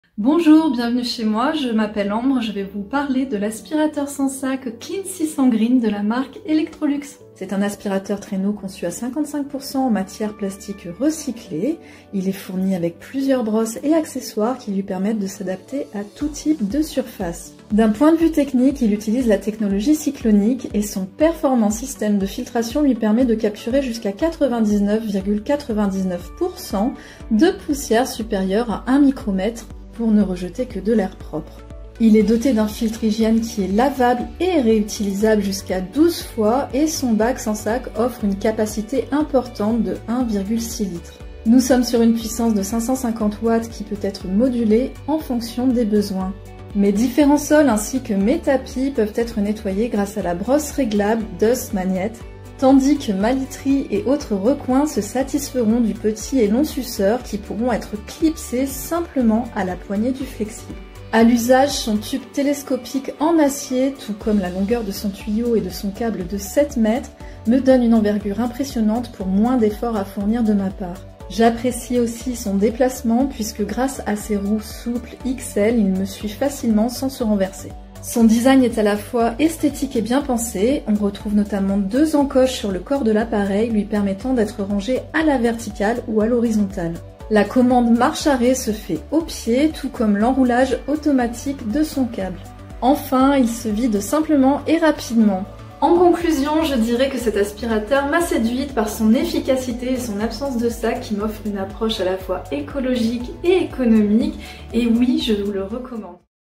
Aspirateur sans sac Electrolux 600 Clean : l'avis de Ambre - Electrolux
Ambre partage son expérience avec l'aspirateur sans sac Electrolux 600 Clean. Avec sa technologie cyclonique innovante et sa conception ergonomique, notre aspirateur sans sac Clean 600 Green combine performances de nettoyage exceptionnelles et vidage simple et rapide. 🍽

► Vidage rapide en 2 étapes du bac à poussière
► Aspiration optimale pendant toute la durée du nettoyage
► Système de filtration en 6 étapes
► Compact pour faciliter le rangement
► Très maniable

Découvrez toutes les fonctionnalités de l'aspirateur sans sac Electrolux 600 Clean

CHAPITRES :
0:07 Présentation du produit
00:47 Démonstration d'utilisation & avis client